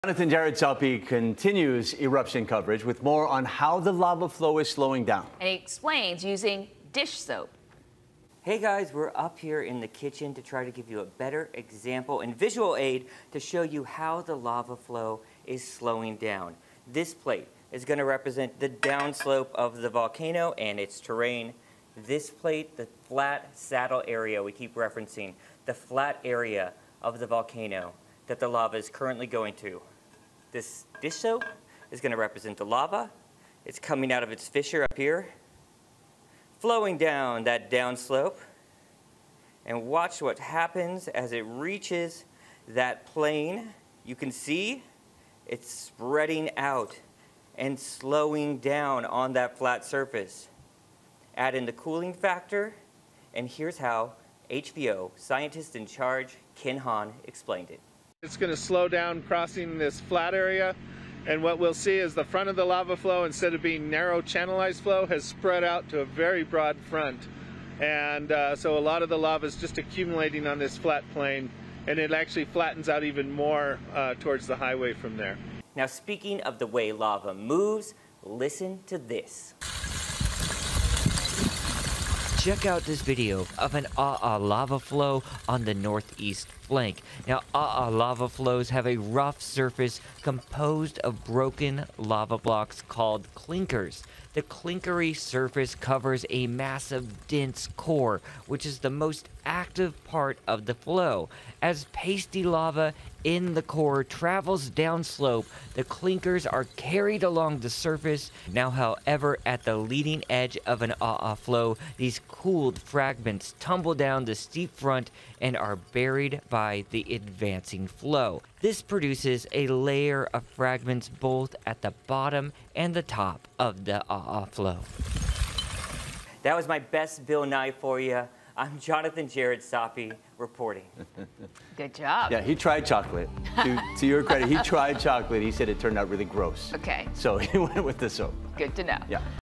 EXPLAINER: Why lava slows down when it moves into flatter area
Jonathan Jared Saupe provides a visual aid explaining how a lava flow slows down when it spreads into flatter area.